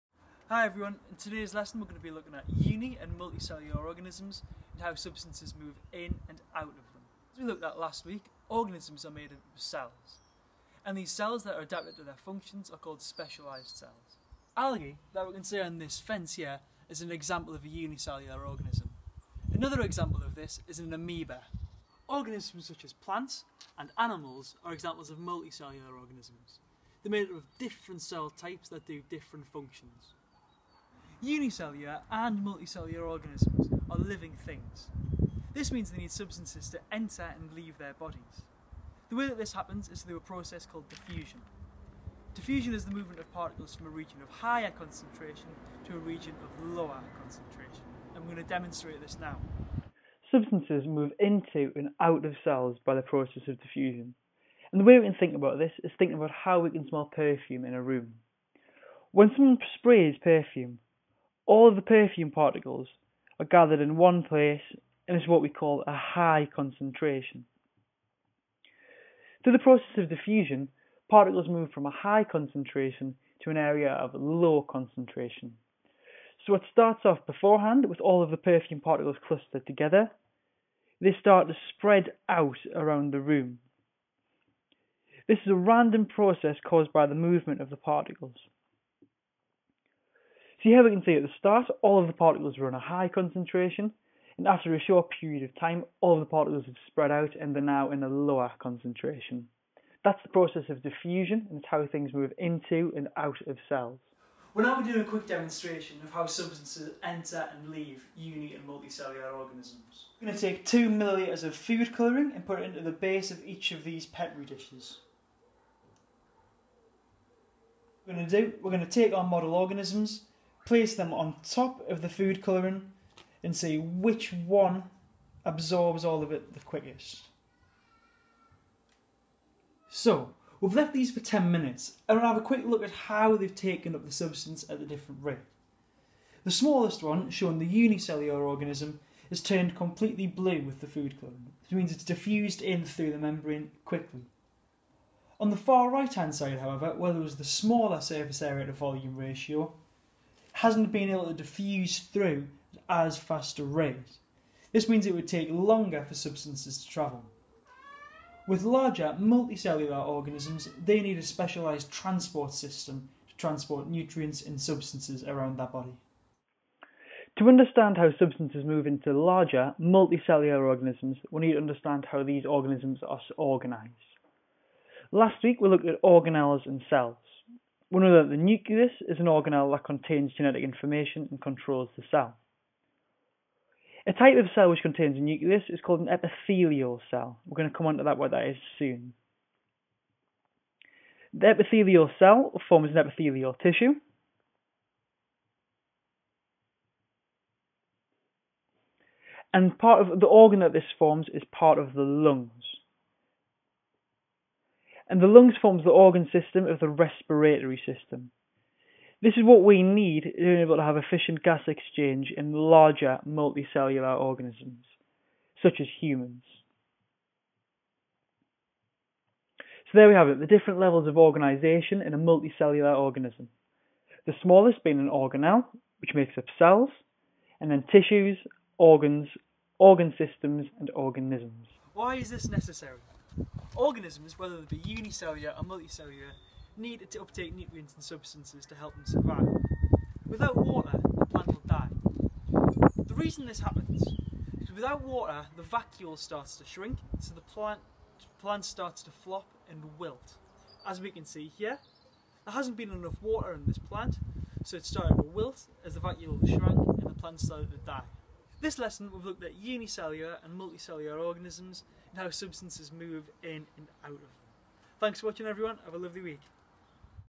KS3 Science - Unicellular and Multicellular Organisms
In today's episode of Archie Science Show, Mr Keedy is discussing the different types of unicellular and multicellular organisms. He is also investigating diffusion.

Happy sciencing!